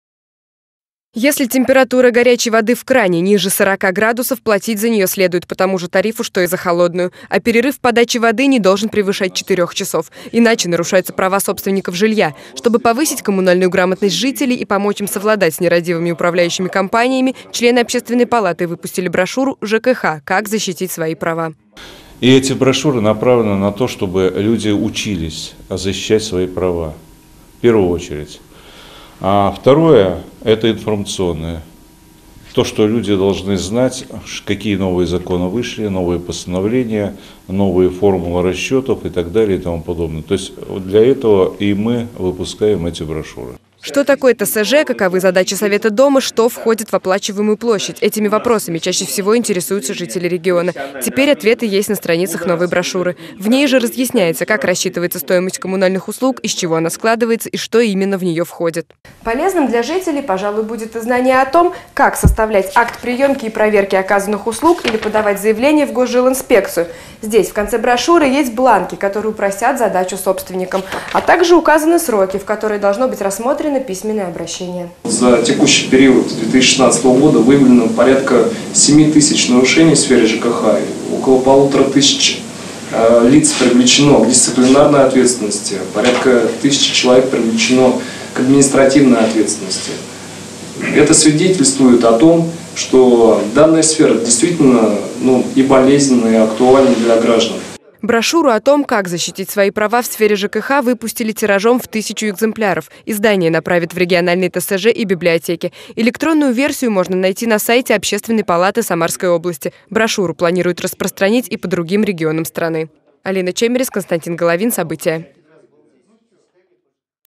Общественная палата Самарской области выпустила брошюру "ЖКХ:как защитить свои права?"
Коммунальный ликбез.  В период отопительного сезона температура воздуха должна быть не ниже 18-ти градусов в обычных комнатах и не менее 20-ти в угловых. Отклонения от нормы допустимы только в ночное время. Об этом следует знать всем собственникам жилья. Чтобы уметь защитить свои права. Специально для этого члены общественной палаты выпустили познавательную брошюру.